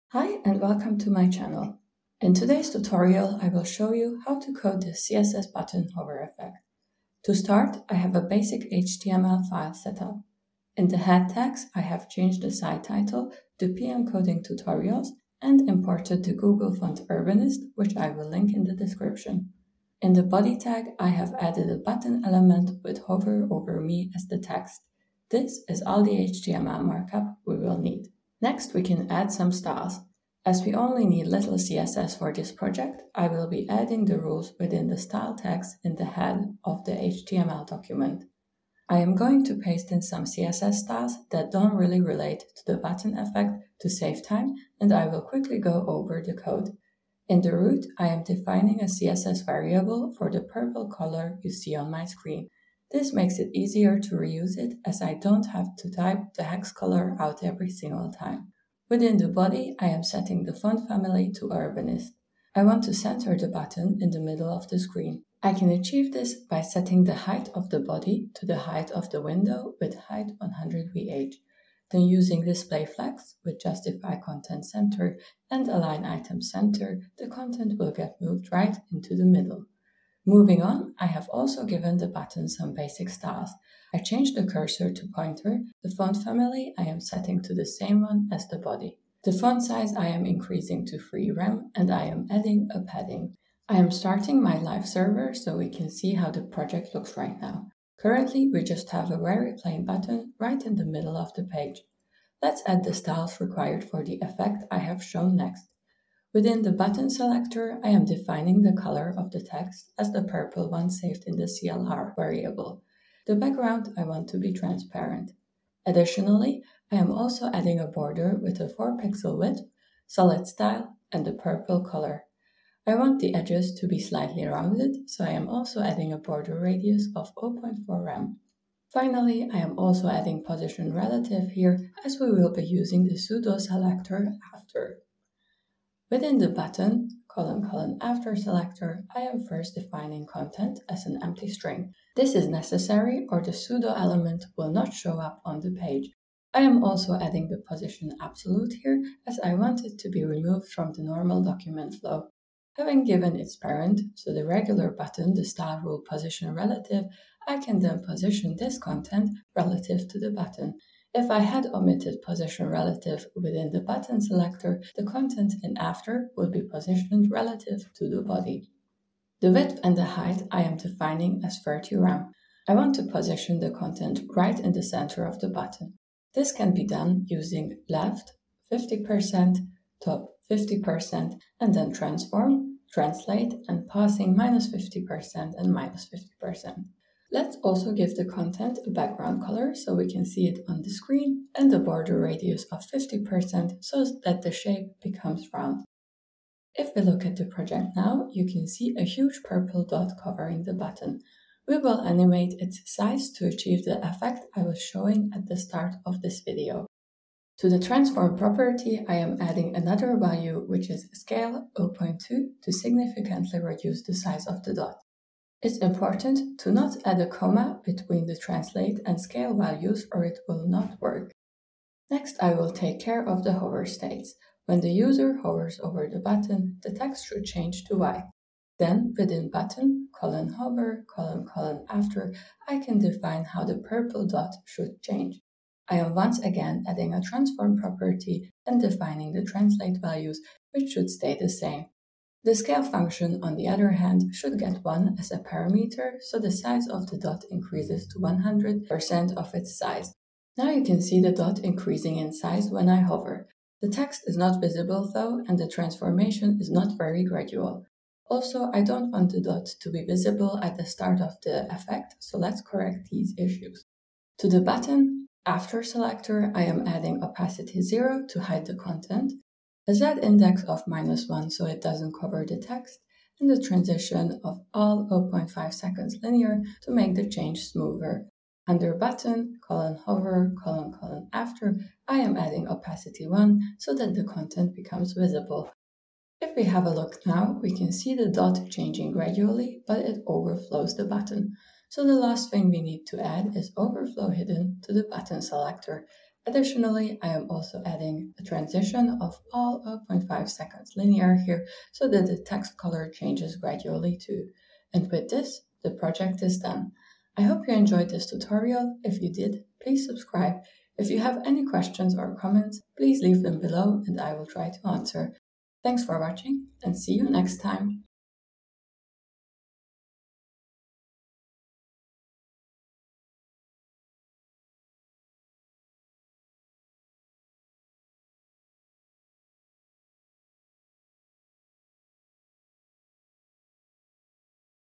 CSS button hover effect tutorial using CSS pseudo elements
In this CSS hover effects tutorial, I will show you how to use CSS pseudo selectors and CSS pseudo elements to create a unique hover effect for your buttons.

I am also on social media, so feel free to connect and reach out: